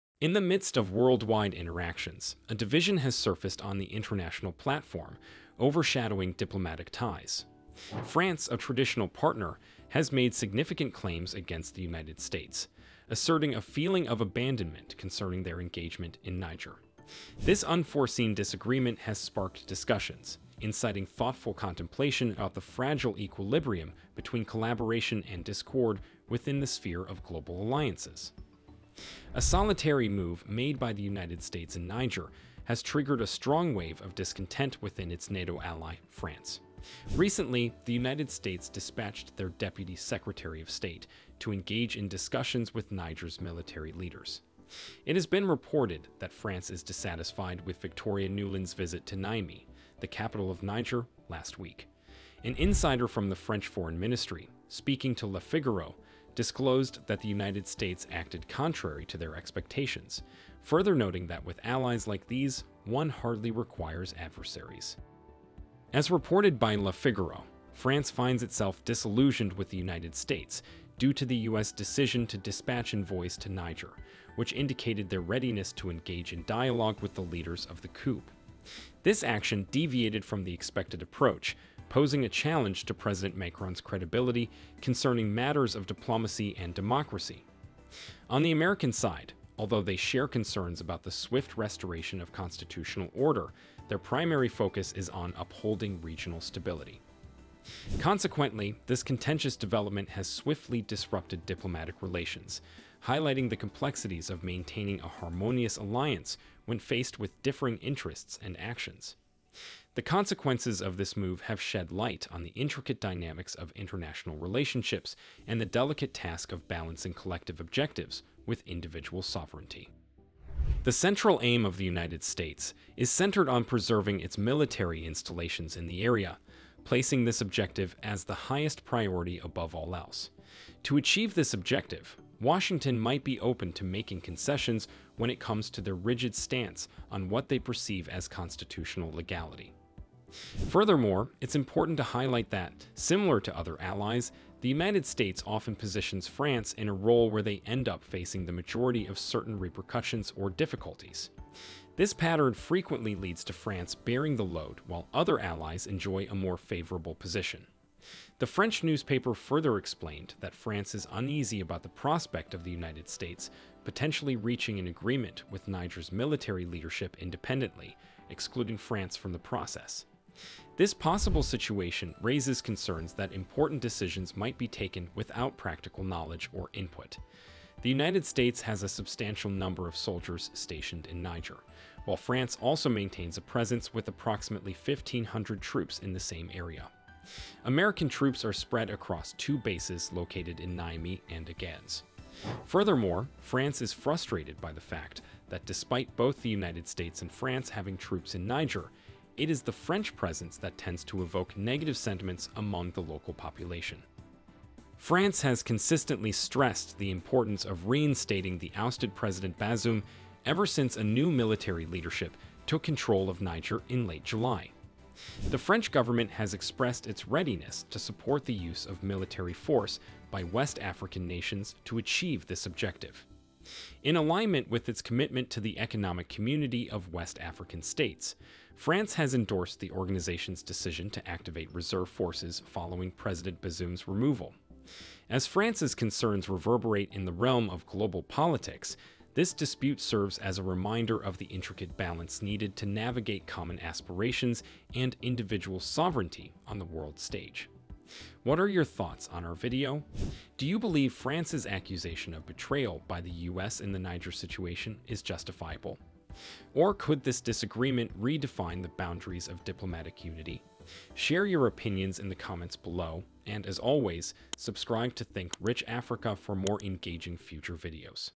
France Is FURIOUS With The United States For BETRAYING Them In Niger Coup
According to reports, France is furious with the United States for sending Victoria Nulland, its deputy secretary of state, to Niger to meet with the military authorities. Paris doesn't need enemies when it has allies like Washington, according to a report in the French newspaper Le Figaro, which cited officials in the foreign ministry. The source added that the US did things that France could not possibly have dreamed of. See this video for more information.

Welcome to THINKRICH AFRICA the community which brings to you entrepreneurial, business, and personal development content to inform, motivate and inspire you!